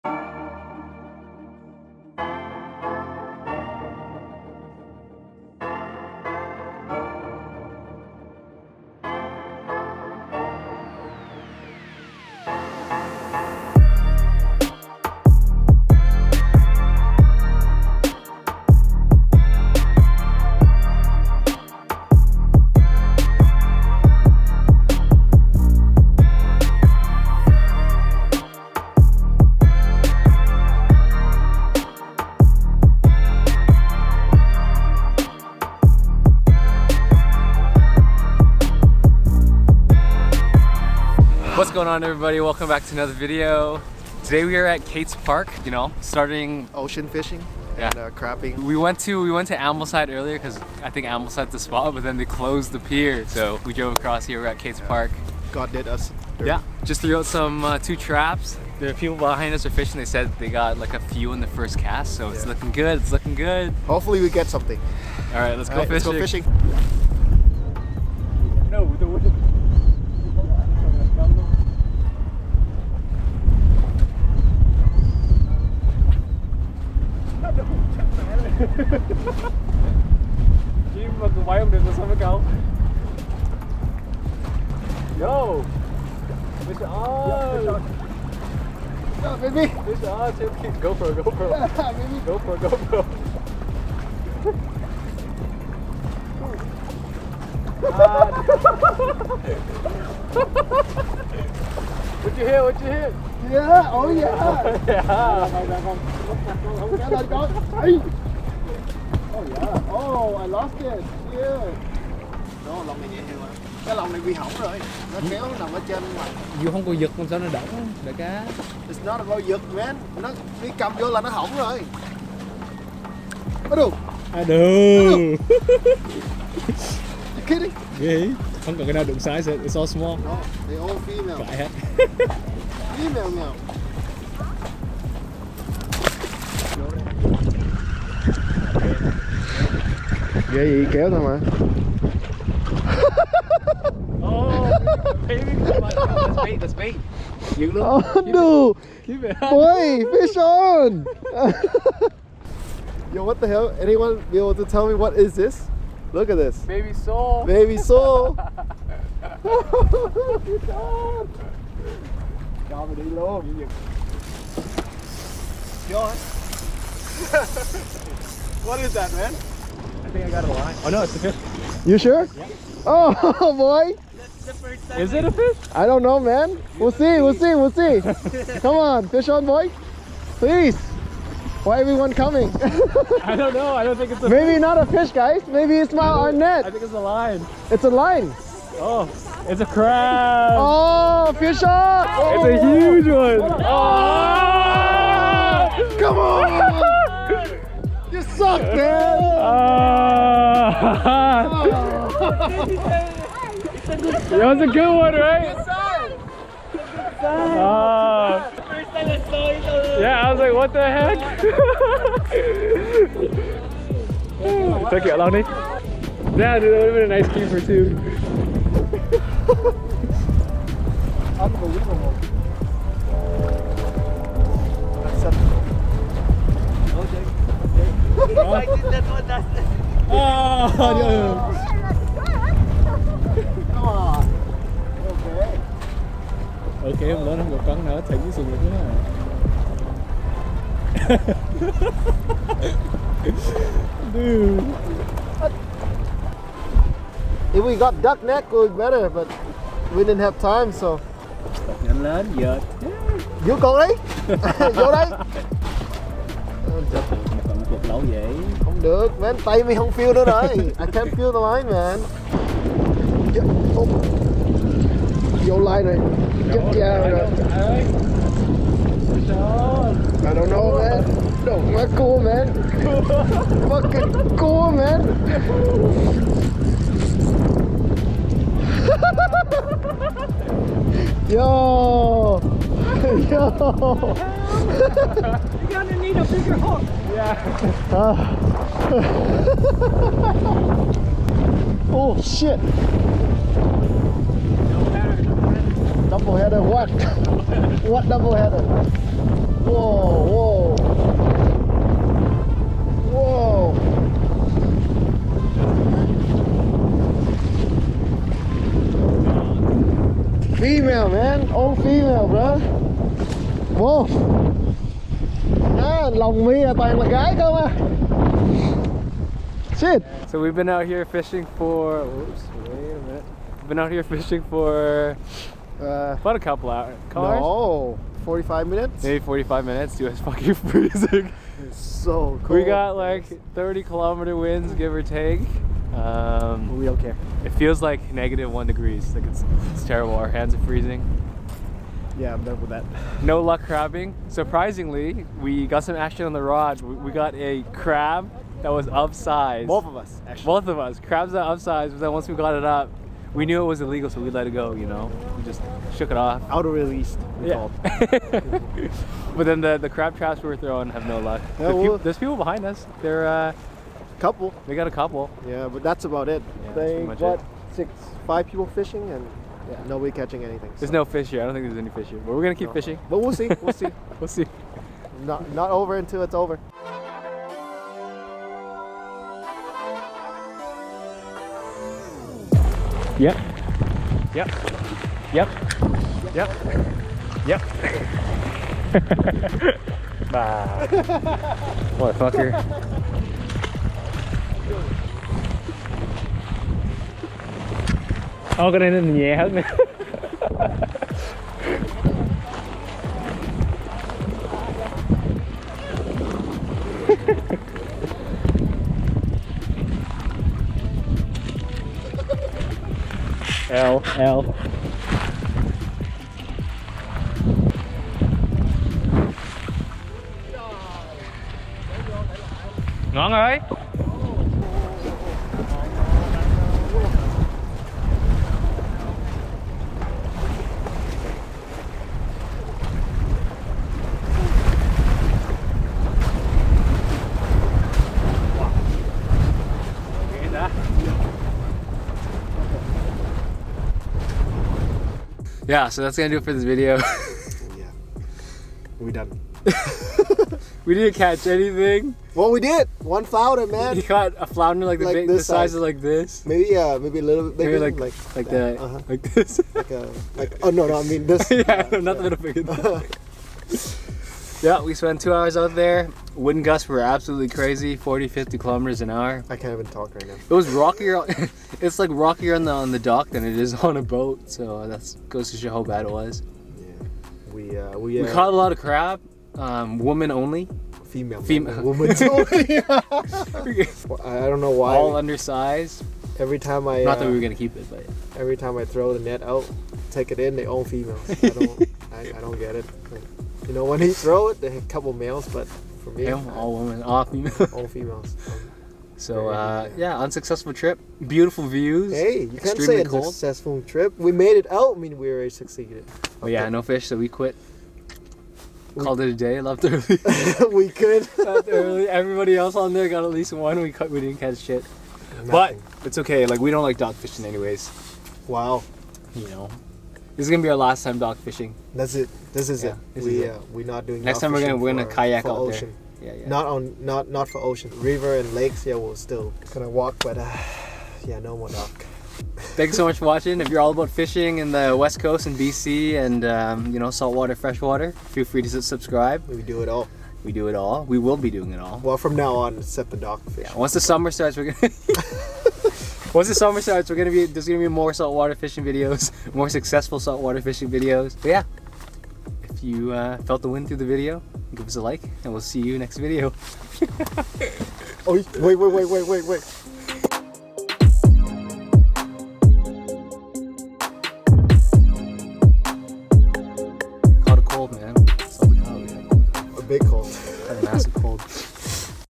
Dock Fishing and Crabbing at Cates Park | EP6
We decided to switch things up and try dock fishing and crabbing at Cates Park in North Vancouver. All we caught was a cold 🤣 We got SKUNKED :(